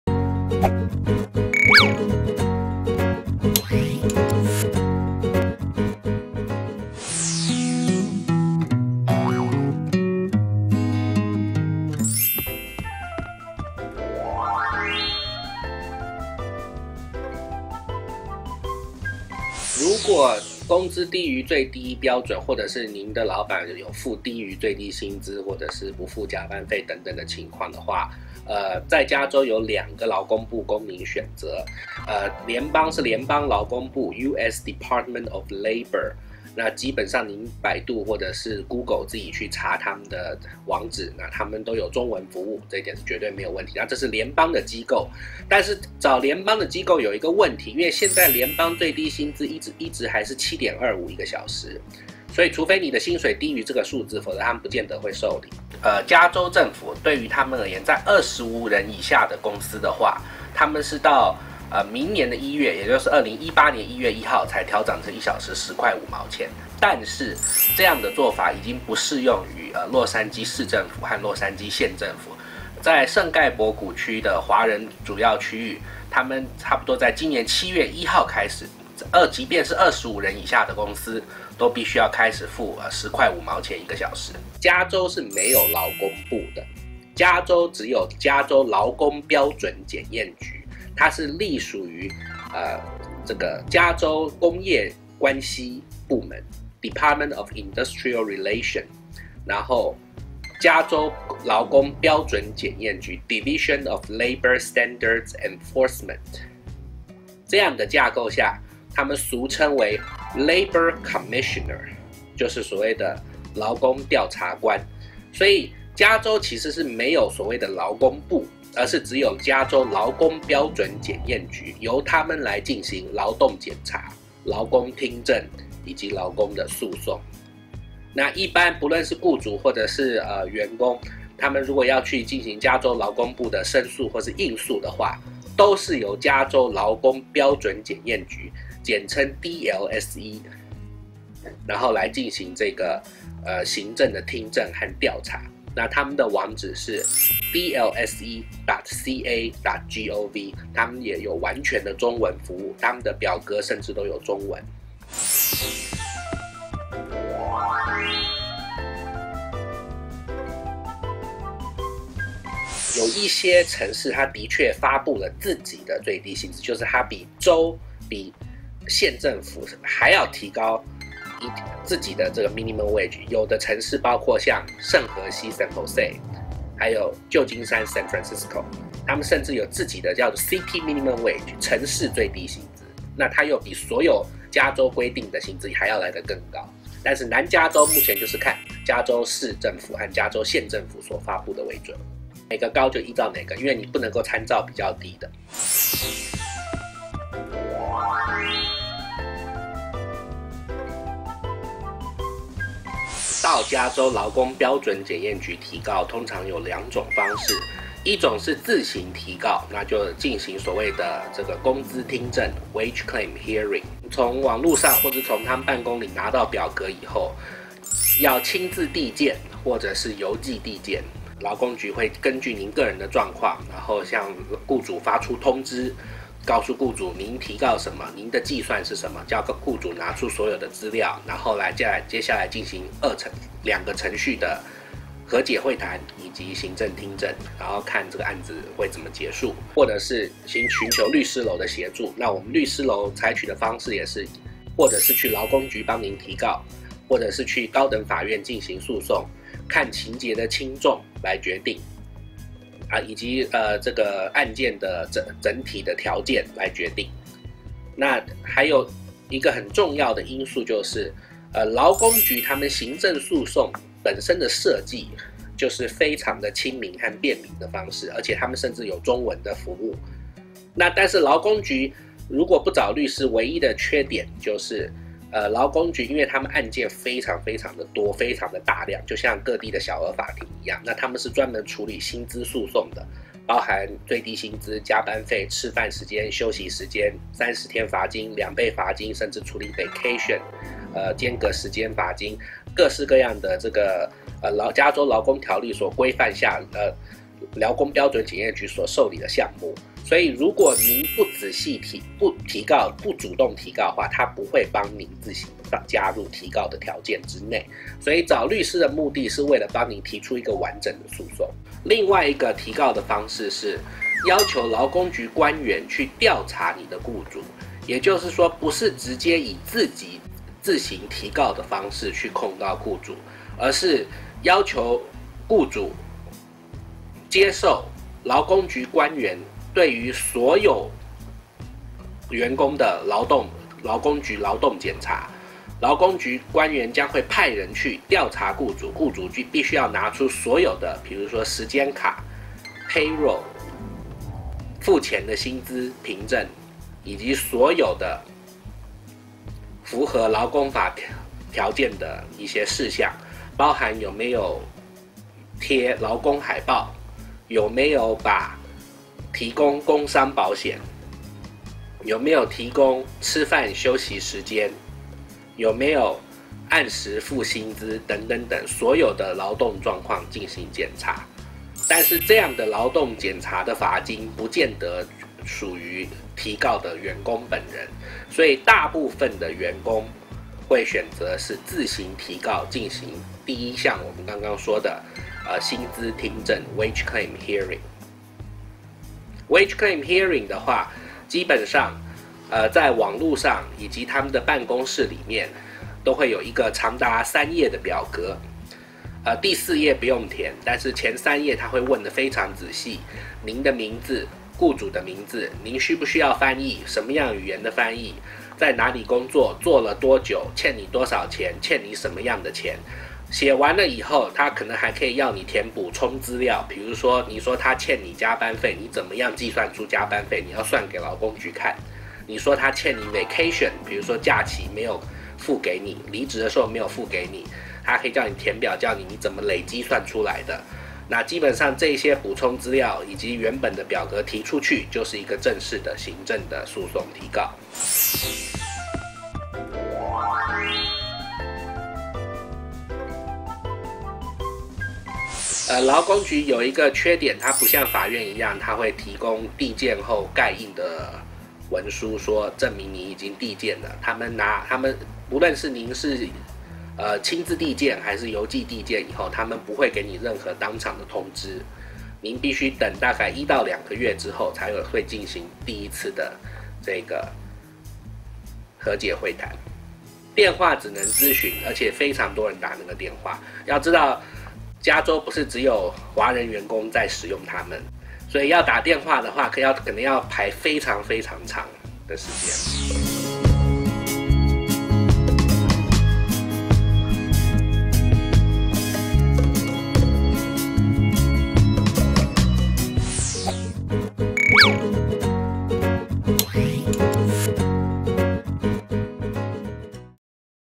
工资低于最低标准怎么办/The pay is below the minimum standard
在美国，当劳工的工资低于最低标准或其他权益受到侵害时，应该向哪些部门寻求帮助呢？如何做才能合法维护自己的权益，这期How视频告诉你答案。
联邦和加州最低工资标准：
如果工资低于最低标准或者是老板支付低于最低薪资标准的工资、不支付加班费等情况，在加州有两个劳工部可以选择：

美国联邦劳工部
美国联邦劳工部United States Department of Labor，美国联邦劳工部网站有中文服务。
找联邦劳工部有一个问题，因为只有薪水低于联邦最低标准7.25每小时（当前标准）美国联邦劳工部才会受理。加州政府规定2018年1月1日加州政府将25人以下的公司最低工资调整为10.5美金每小时。但是这并不适用于洛杉矶县政府和洛杉矶市政府，在圣盖博谷区的华人区域在2017年7月1日开始，25人以下的公司最低工资就调整为10.5美金每小时。

美国联邦劳工部的标准可能不适用于当地政府标准。

加州劳工标准检验局
加州没有劳工部，相关机构为加州劳工标准检验局（DLSE），隶属于加州工业关系部Department of Industrial relation，俗称为劳工调查官Labor Commissioner，雇主或员工进行加州劳工部的申诉应诉都是由加州劳工标准检验局进行行政听证和调查。

员工该参照哪个最低工资标准？
有一些城市发布了自己的最低薪资标准，比州、县政府更高，比如圣荷西、旧金山等。但目前南加州以州政府和县政府最低工资标准中较高的为准。

员工如何提告？
提告方法一 自行提告
进行工资听证Wage claim hearing，从网络上下载或劳工局办公室领取提告表格，填写后必须要亲自递交或邮件递交。劳工局会根据状况向雇主发出通知，告诉雇主员工提告什么，要求雇主拿出所有的资料，接下来进行和解会谈和行政听证，然后看如何结案。
进行工资听证Wage claim hearing需要填写一份表格，详细填写前三页的内容，包括：员工资料、雇主资料、是否需要翻译、在哪里工作、工作多久、雇主欠多少钱，欠什么钱等。另外还有补充资料，比如雇主欠员工加班费，那么需要写明如何计算的加班费。把这份表格填写好提交出去就是一份正式行政诉讼提告。

提告方法二 寻求律师的帮助
律师会视情况去劳工局提告或去最高法院提出诉讼。
劳工局行政诉讼十分亲民，会有中文电话以及中文表格。但有一个缺点就是因为劳工局的案件非常的多，提告人不主动索要赔偿，劳工局不会主动增加赔偿条款，所以找律师可以最大限度维护提告人权益。

按照规定劳工诉讼只能拿索赔金额的15%，又不要还是找律师帮助提告诉讼。

提告方法三 要求劳工局官员调查雇主
劳工局官员的调查是全方位的调查，雇主接受调查需要拿出所有的材料，比如时间卡、工资单、支付薪资凭证、有没有贴劳工海报、有没有提供工伤保险、有没有提供吃饭休息时间，有没有按时付薪资等所有劳动状况的检查。

但是这种劳动检查后对老板处罚的罚金很可能不会落实给提告者，所以大部分员工会选择方式一，自行提告。

如何与劳工局打交道？
劳工局不会提供递减盖印的文书证明已经递件，加州劳工标准检验局收件后需等待1-2个月时间，进行第一次和解会谈。

电话仅提供咨询相关信息的服务且实用性不是很好，可能要等非常久的时间。